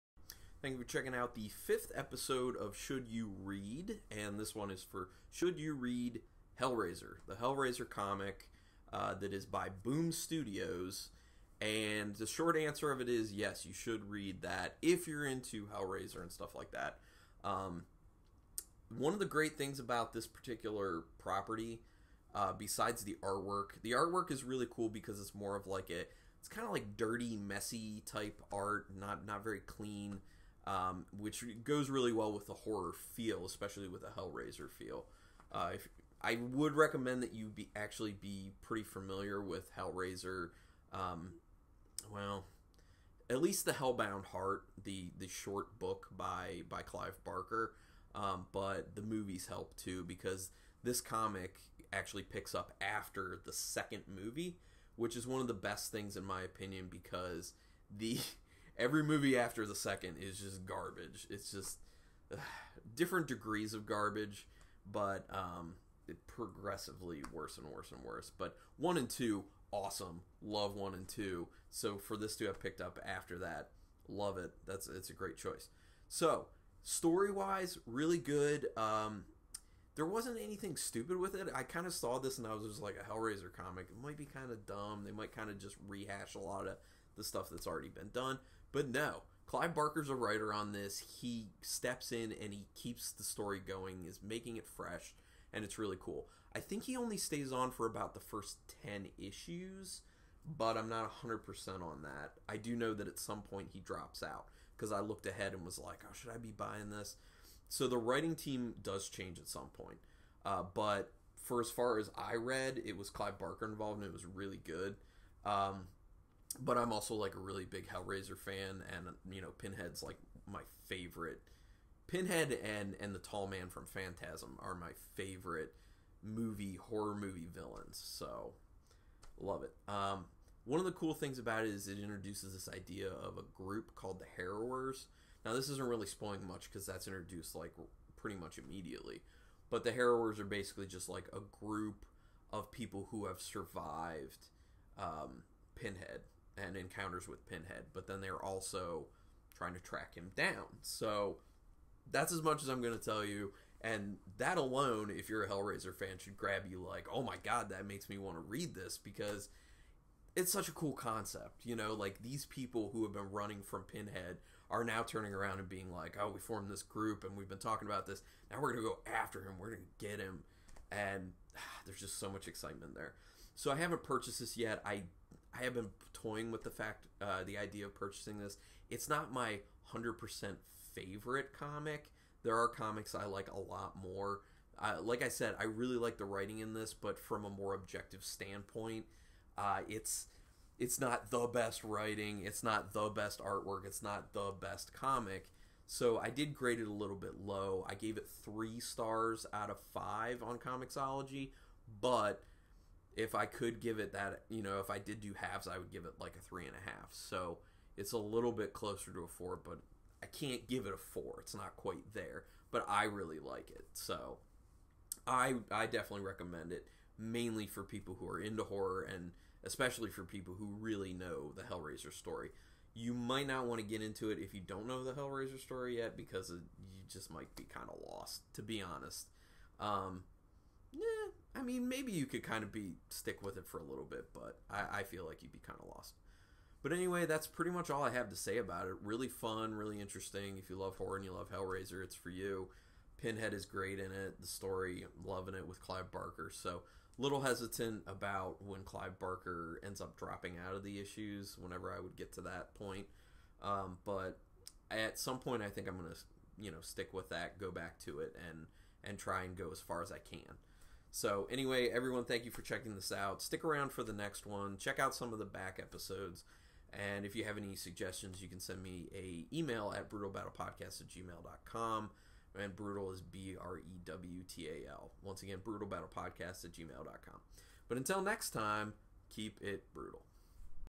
Should You Read... Hellraiser?
This video review answers the question: should you read the comic Hellraiser, by Boom! Studios?